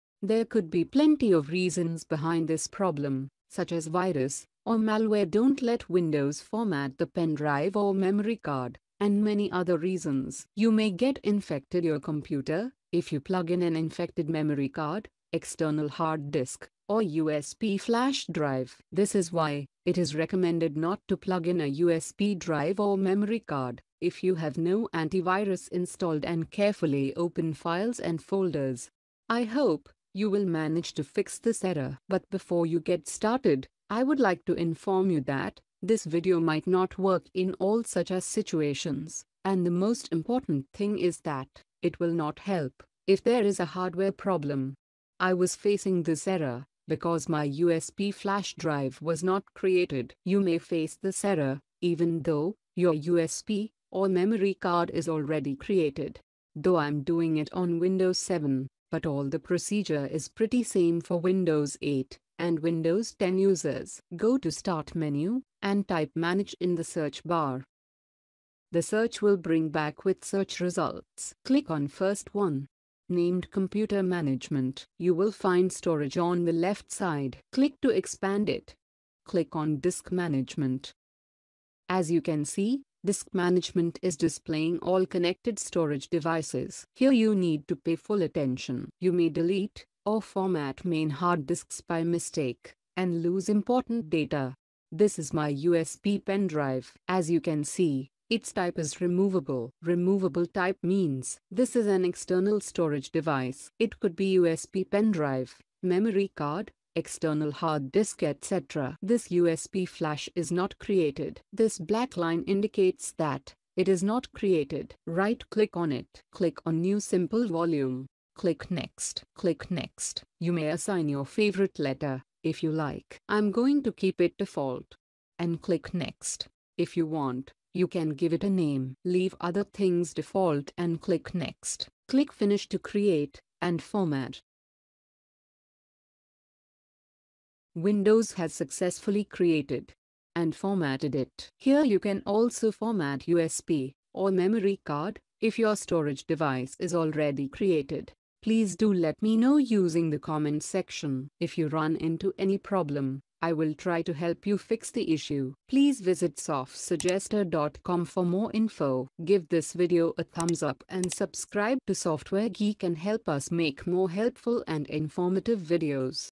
How To Fix Windows Was Unable To Complete The Format USB/Card/Hard Drive On Windows 11/10/7/8?
Windows Was Unable To Complete The Format Windows 8
This video on Windows Was Unable To Complete The Format fix is being made on Windows 7. But You can follow given instructions to fix Windows Was Unable To Complete The Format on Windows 11, Windows 10, Windows 8, Windows XP, Windows Vista as well.
This video can be used for hard drive, external hard drives, SD memory card, USB Flash drives and other internal and external storage devices.
We hope You will manage to fix this error. But before You get started, I would like to inform you that this video might not work in all such as situations and the most important thing is that It will not help if there is a hardware problem.